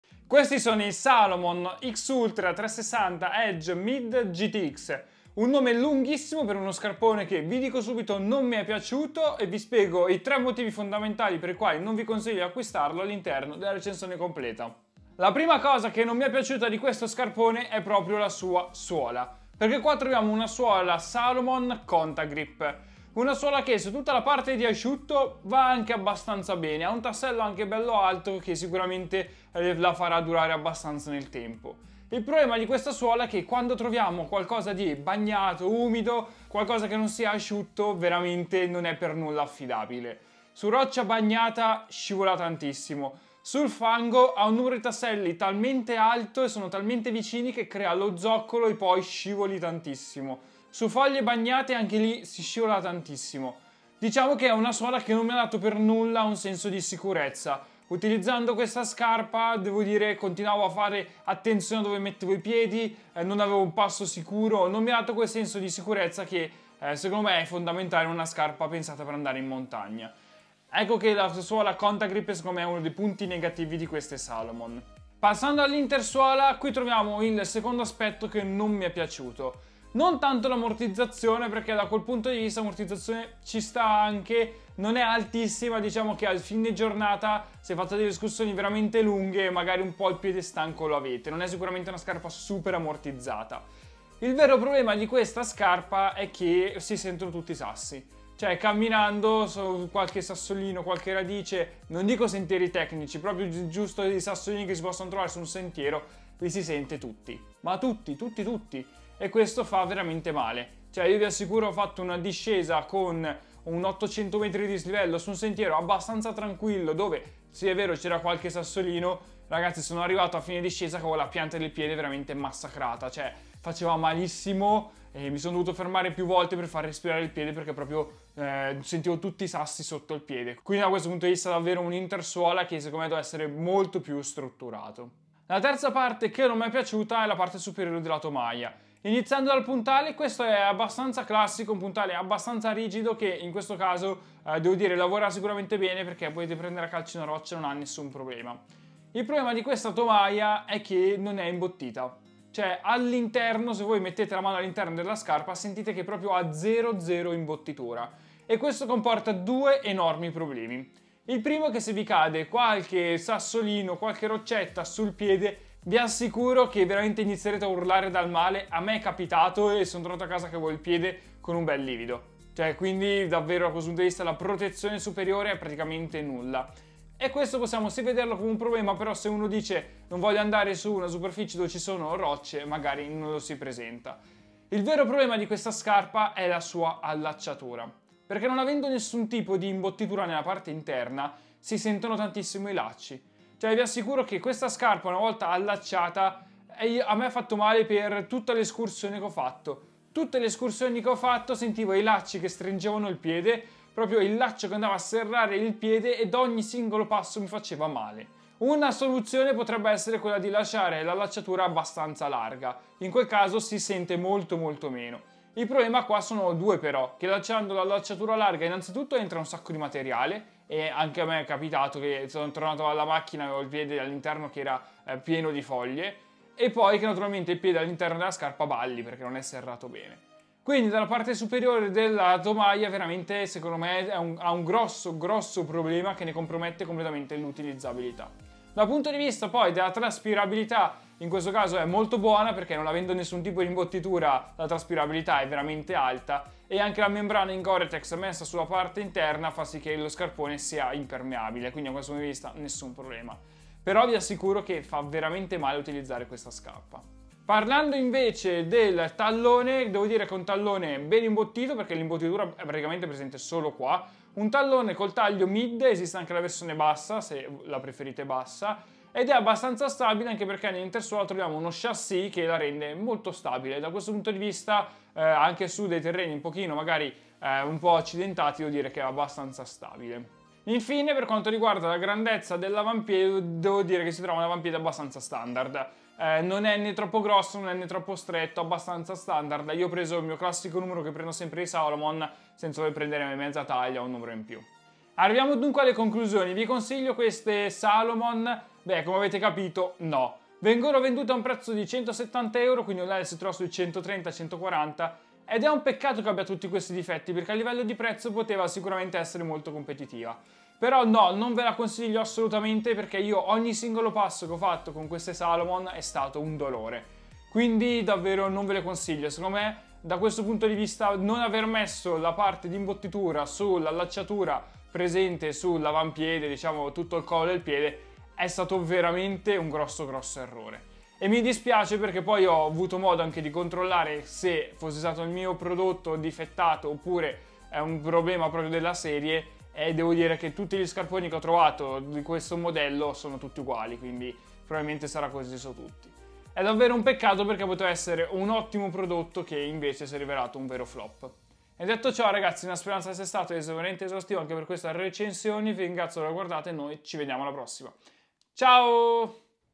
BOCCIATI 👎 - Recensione Salomon X Ultra 360 Edge Mid GTX
Oggi parliamo dei nuovi Salomon X Ultra 360 Edge Mid GTX, uno scarpone da trekking che non mi è piaciuto sotto molti punti di vista.
Buona visione!